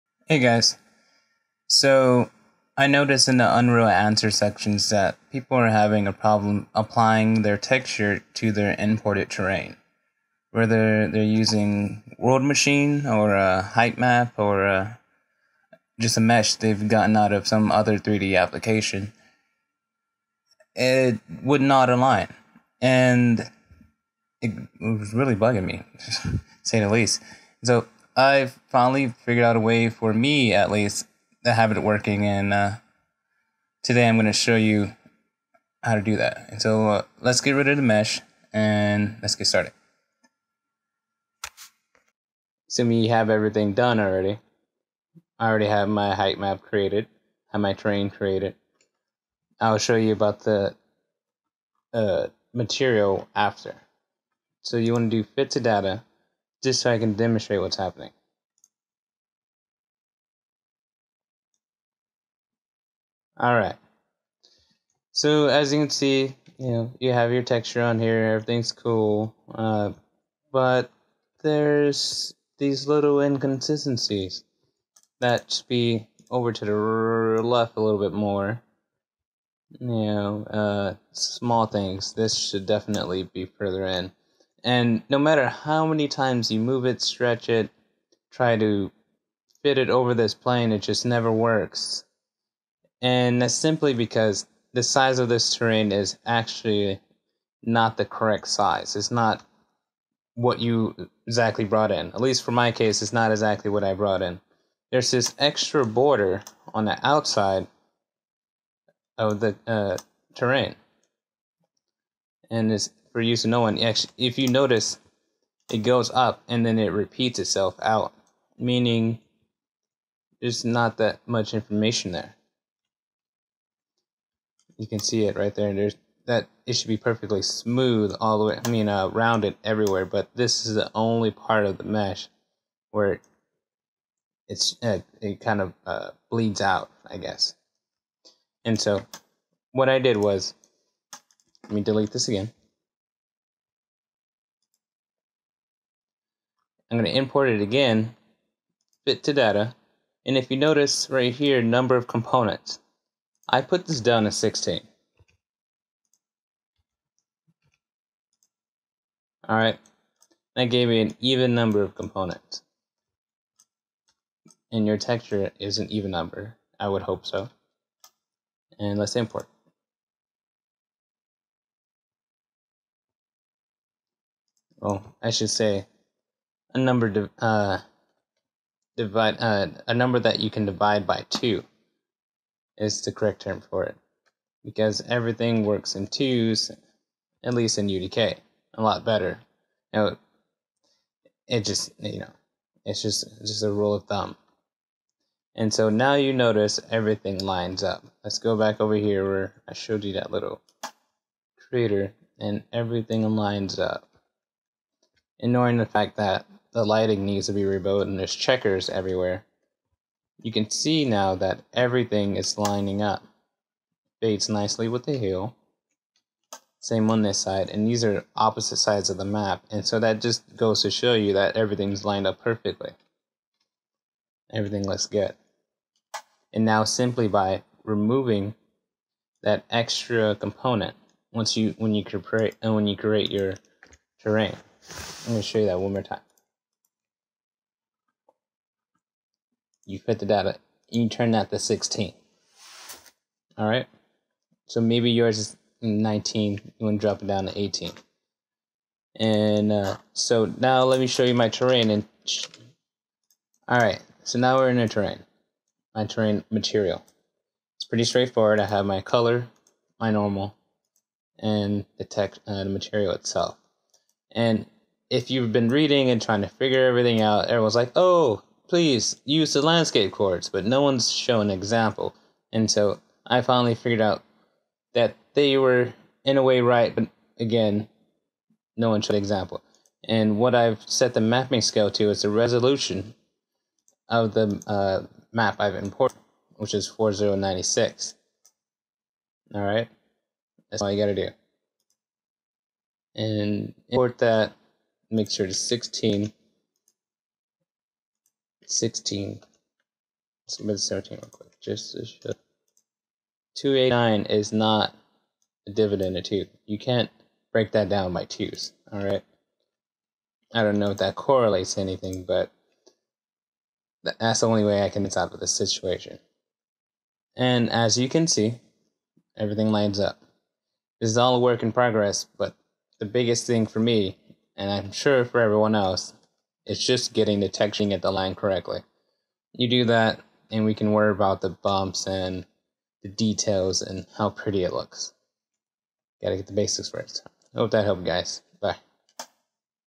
Apply Material to Terrain : Unreal Engine 4
In this Tutorial I explain how to apply a material to custom terrain imported into Unreal Engine 4. I also explain how I used LandscapeCoords node with the Material Editor.